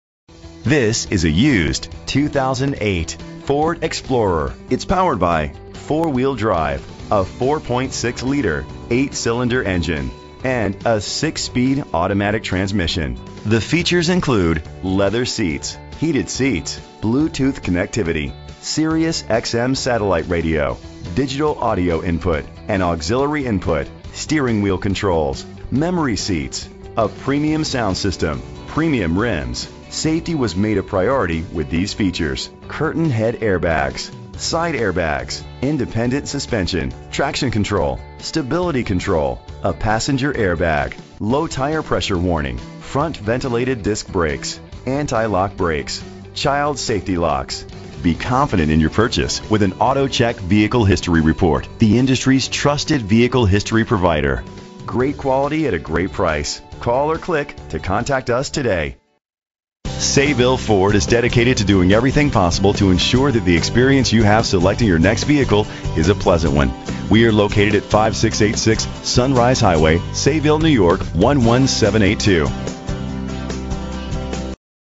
2008 Ford Explorer - Sayville NY
Phone:
Year: 2008
Make: Ford
Model: Explorer
Trim: Limited
Engine: 4.6 liter 8 cylinder 24 valve
Transmission: 6-Speed Automatic
Color: Dark Cherry Metallic
Mileage: 124164
Address:
5686 Sunrise Hwy.